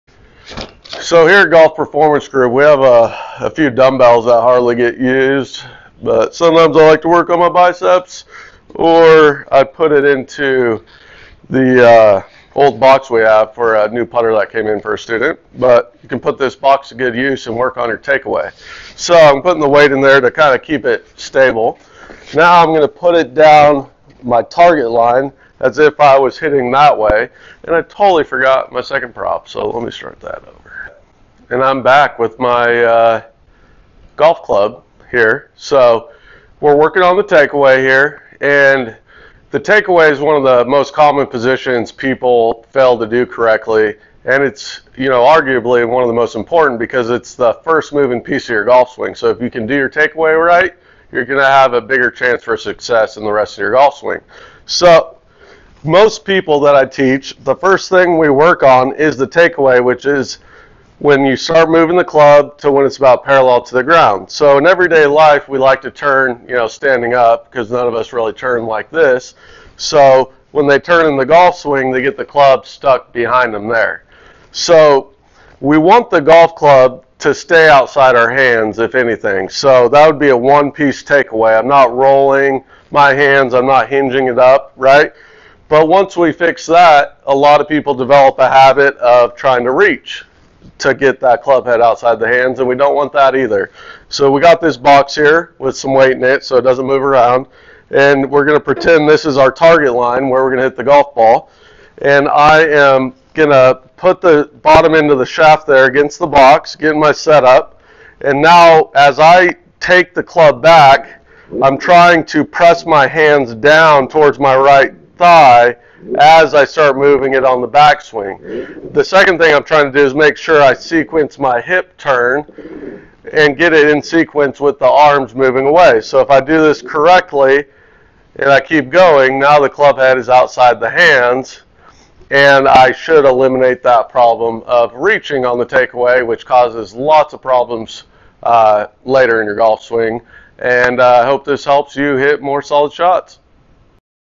Full Swing - Take away drill with box
This drill helps you develop the feel for a one piece takeaway in your full swing.

JJ Wood, PGA Director of Instruction
Golf Performance Group, Northgate Country Club